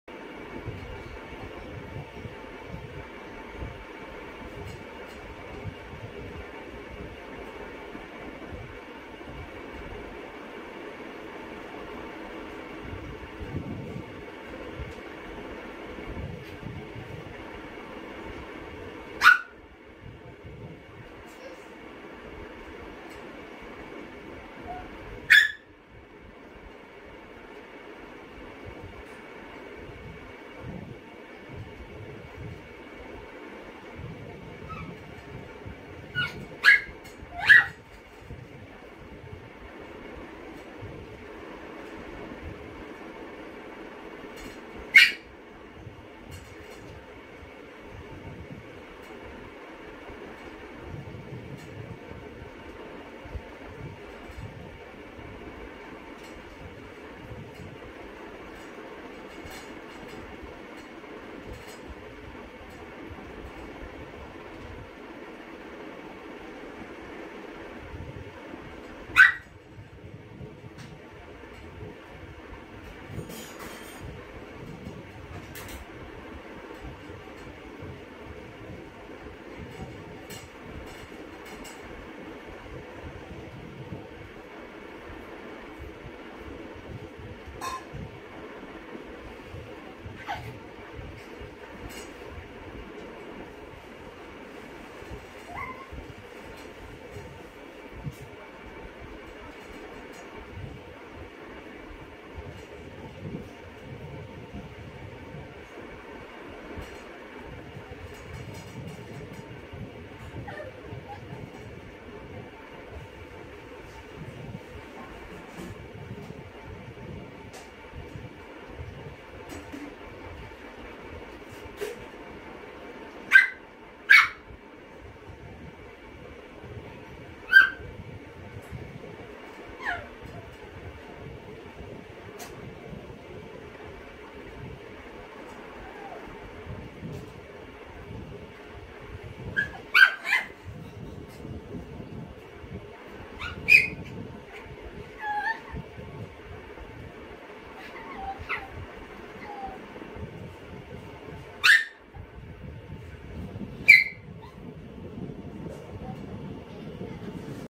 The Chihuahua Ms.Tia Drama Queen#Tomy the Mr.Perfect#
Tia The Drama Queen Chihuahua,Trying to snatch the potato finger from Tomy Mr.Perfect